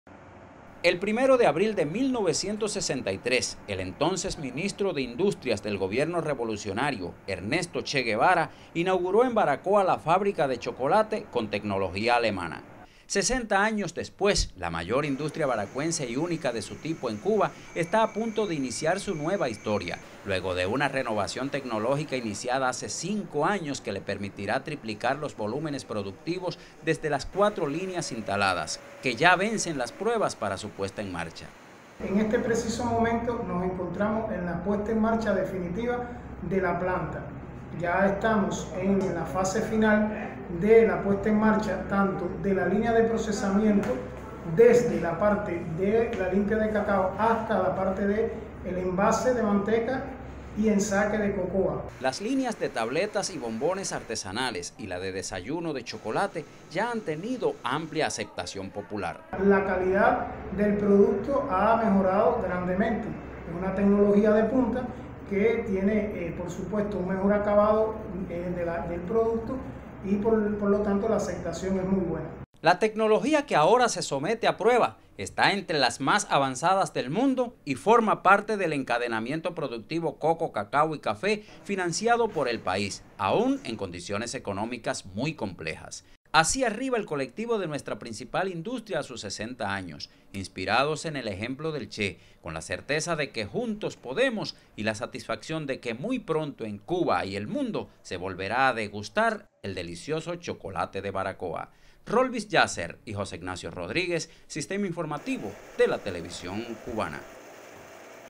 Fábrica de Chocolate de Baracoa: 60 años tributando a la economía y la vida social del territorio
El Primero de Abril de 1963 el Guerrillero Heroico Ernesto Che Guevara fundó la Fábrica de Chocolate de Baracoa. La industria arriba a sus 60 años con una reconversión Tecnológica General que le permitirá mayor eficiencia y calidad productiva